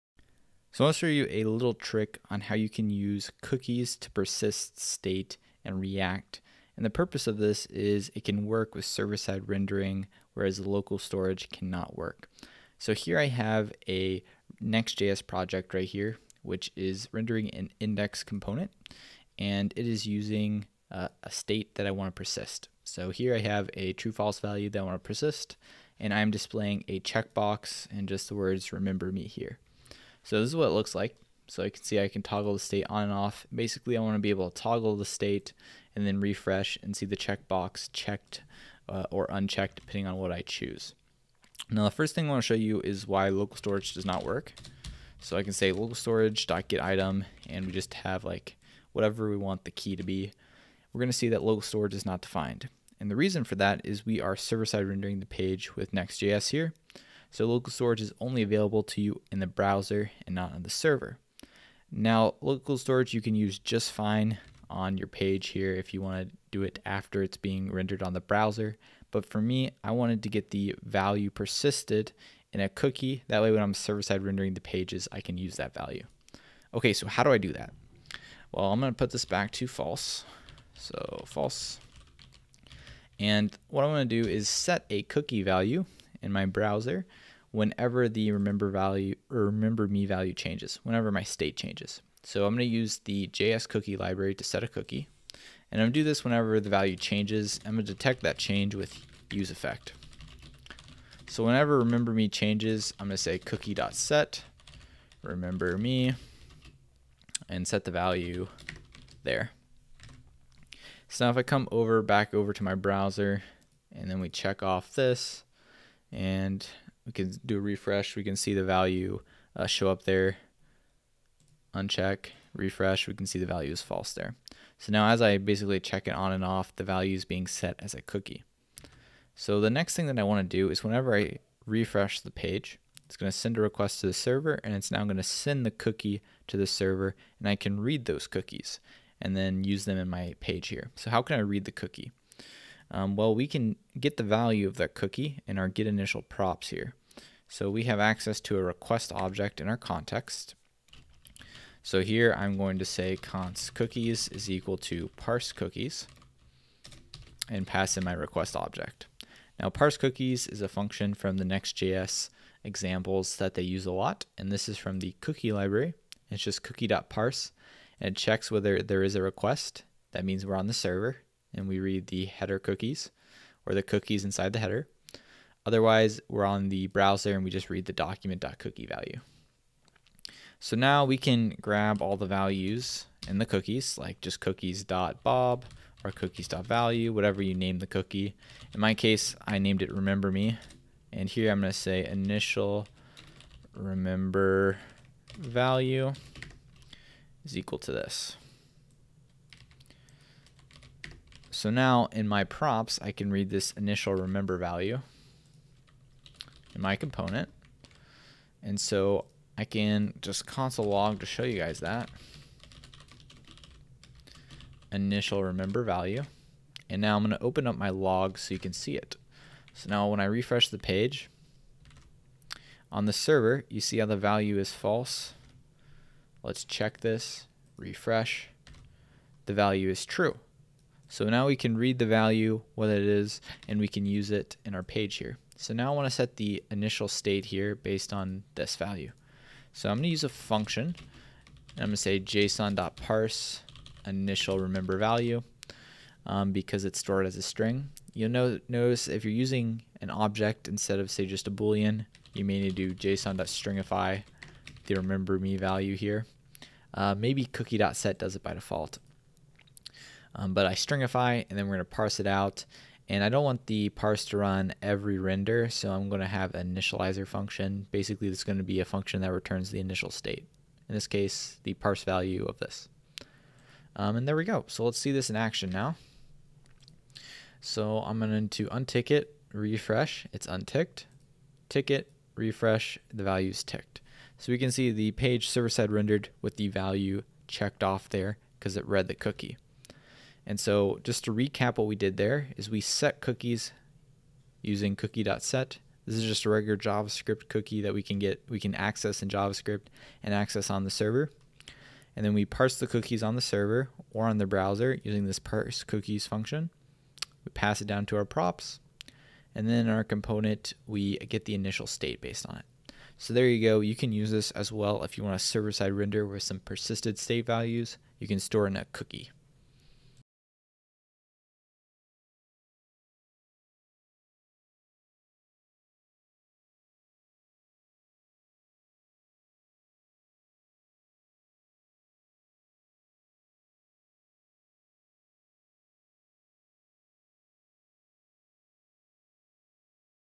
Next.js Persist State with Cookies
Learn how you can persist state in Next.js with cookies.